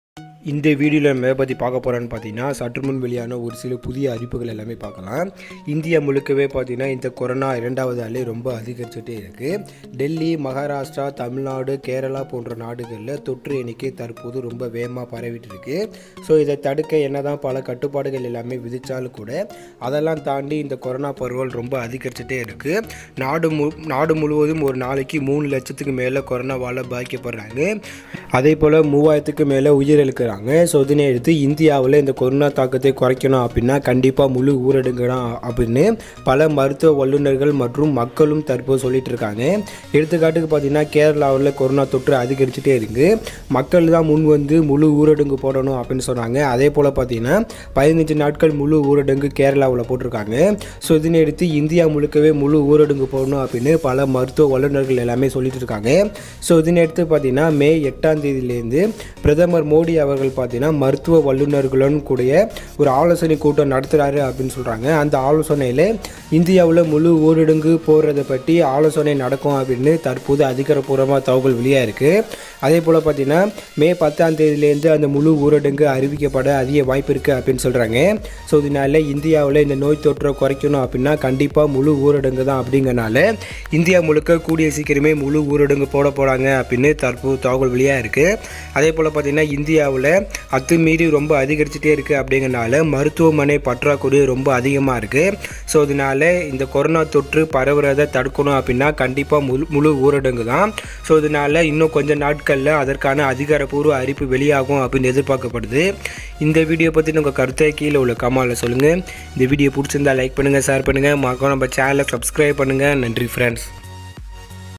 இந்தியாவில் மீண்டும் முழு ஊரடங்கு உத்தரவு அமல் படுத்தப்படும் என பிரதமர் மோடி அறிவிப்பு!Lockdown News
இந்தியாவில் மீண்டும் முழு ஊரடங்கு உத்தரவு அமல் படுத்தப்படும் என பிரதமர் மோடி அறிவிப்பு!Lockdown News | Tamil News | Breaking News | Today Corona Update | Today News | Tamilisam |